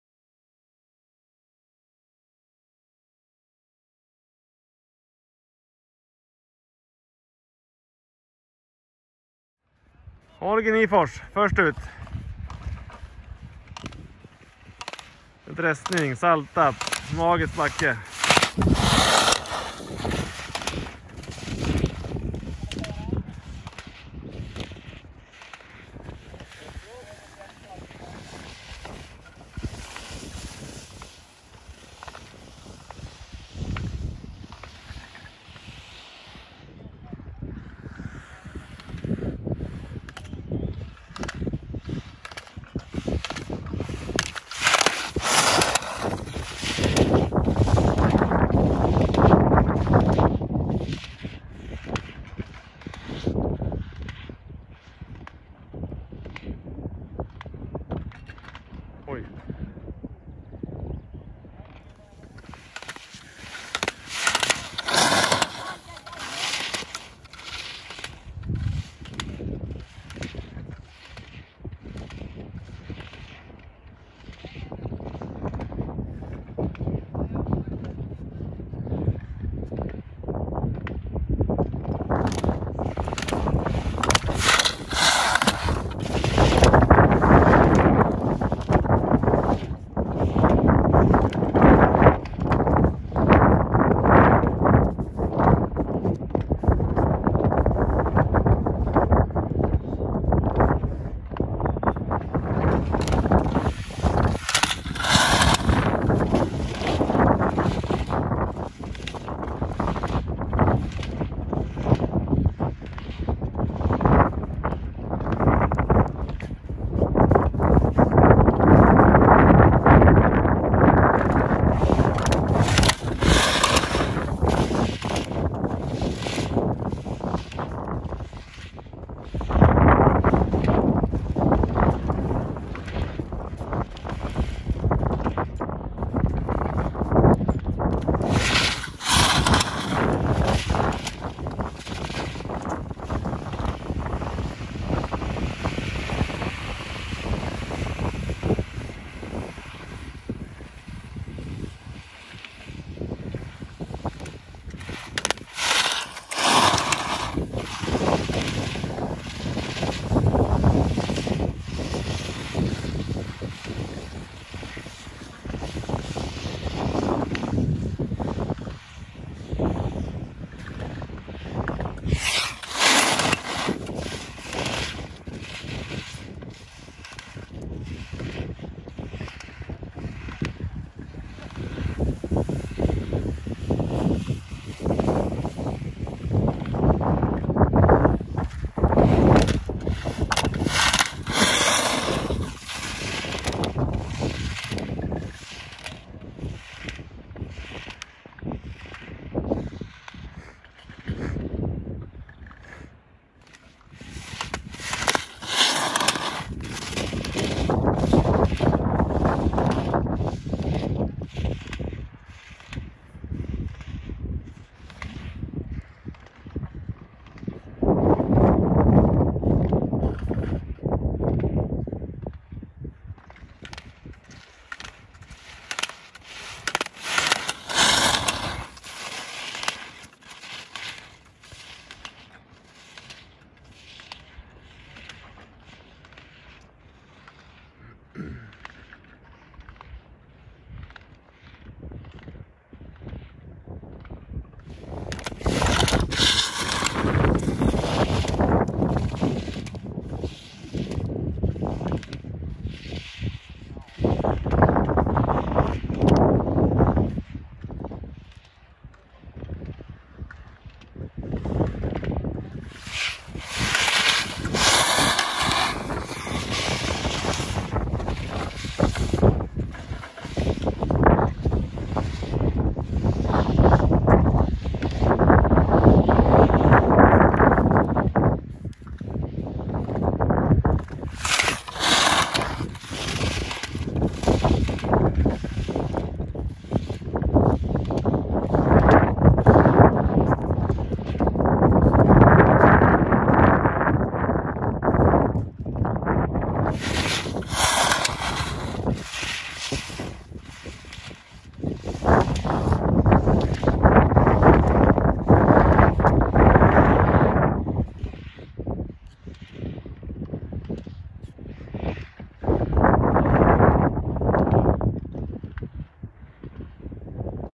Sundsvalls Slalomklubbs U-14 kör en träningstävling i slalom 2020-04-10 åk 1
Bra och kul slalomåkning i Sundsvalls Slalombacke med U-14 gänget i Sundsvalls Slalomklubb